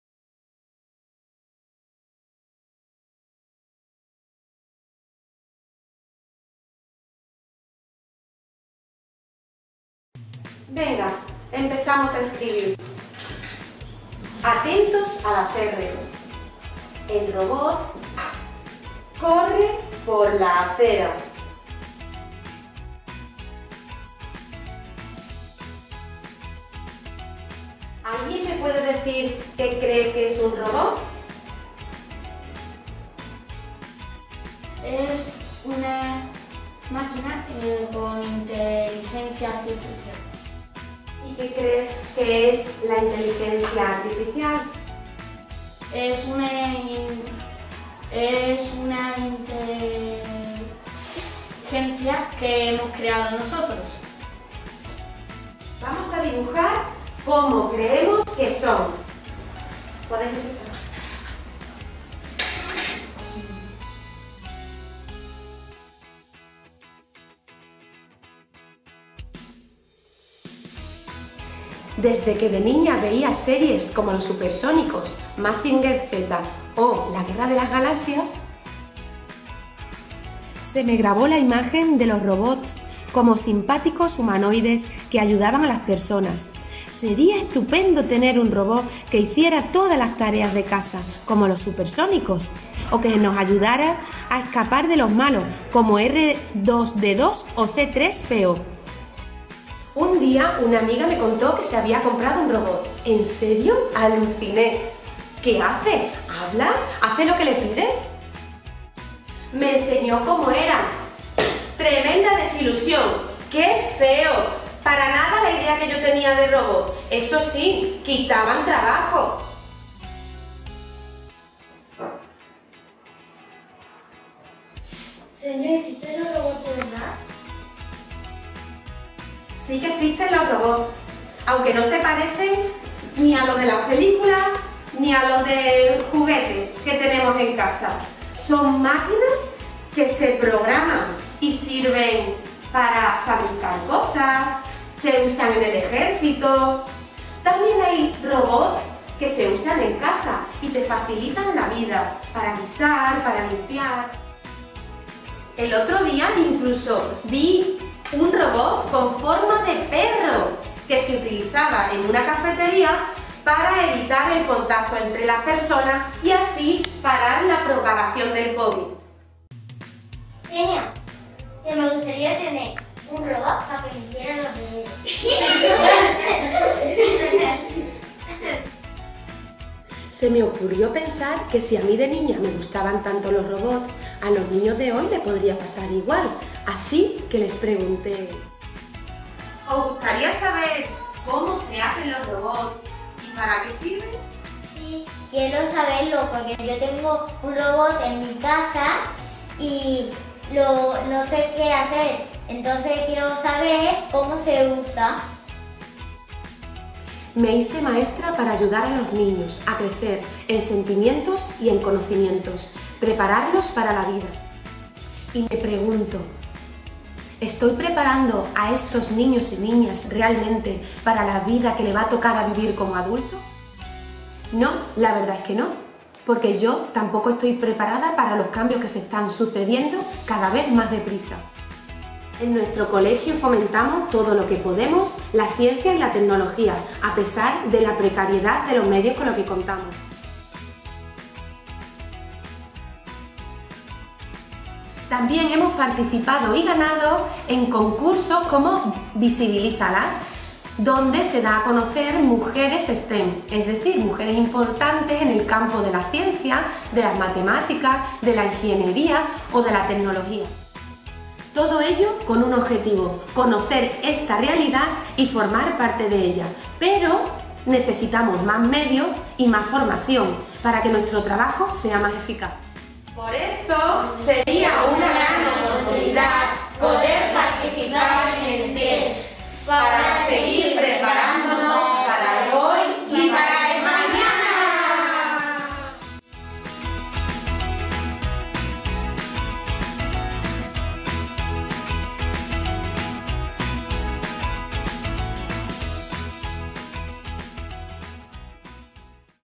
CEIP Huerta del Carmen
Vídeo elaborado para la participación del CEIP Huerta del Carmen en el proyecto RetoTech organizado por la Fundación Endesa